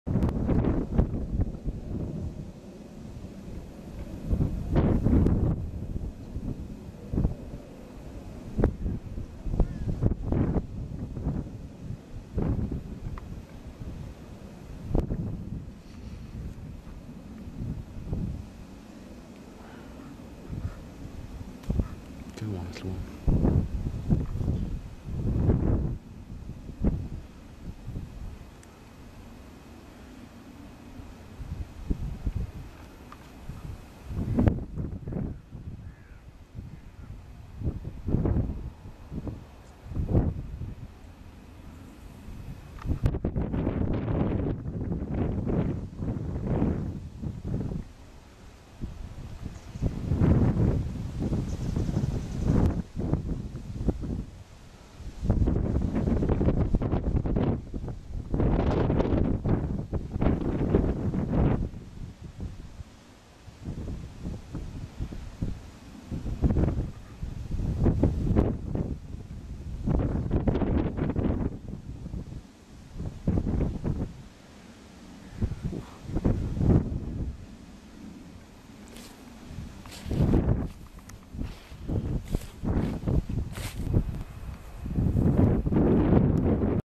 Alpaca Giving Birth
Scarlet delivering her cria,Sienna,June 2007.
Thorpe in the Glebe, Notts